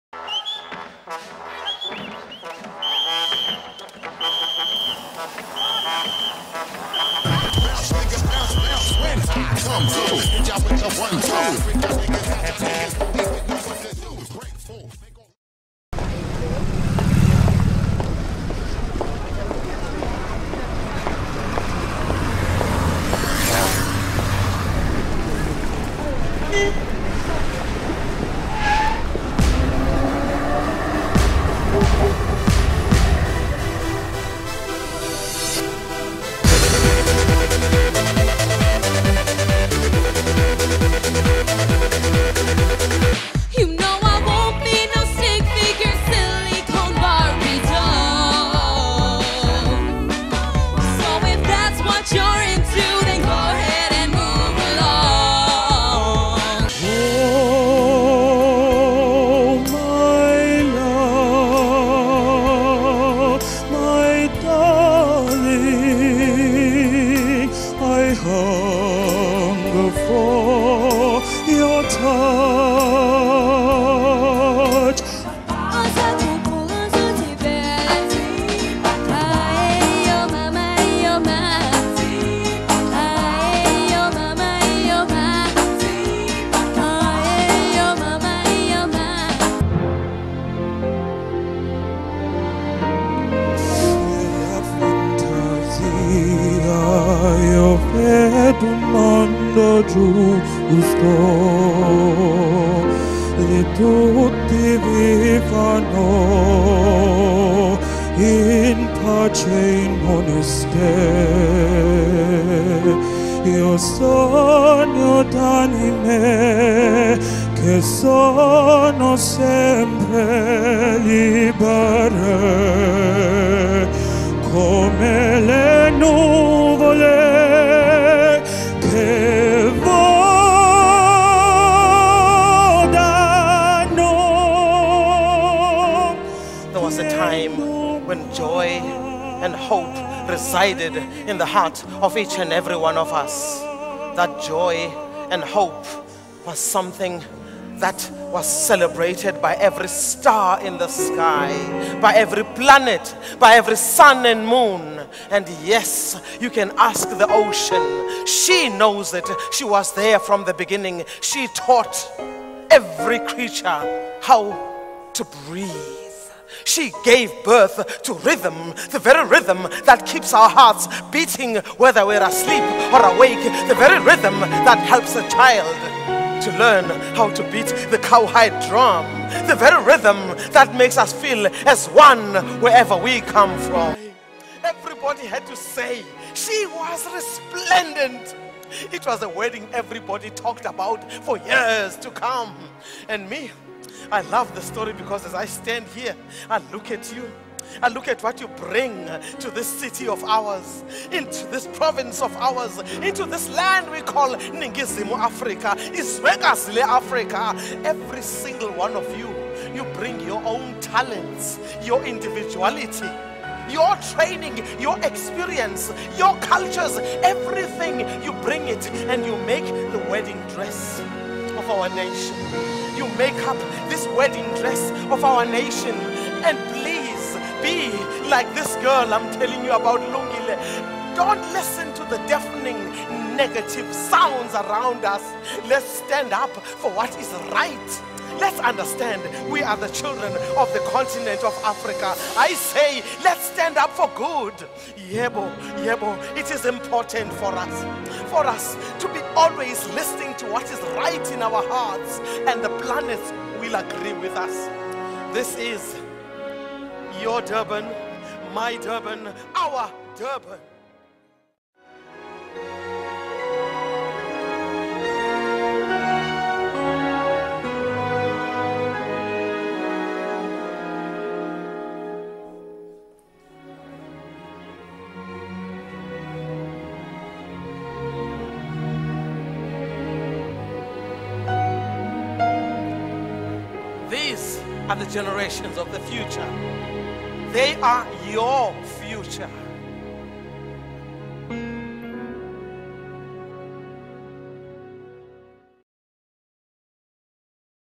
Durban Chamber of Commerce
Edited highlights from Makulu's production at the Durban Chamber's Annual Gala Dinner at Durban ICC.
Technical by Black Coffee Productions
AV by Albet Eloff
Lighting by Mike Broderick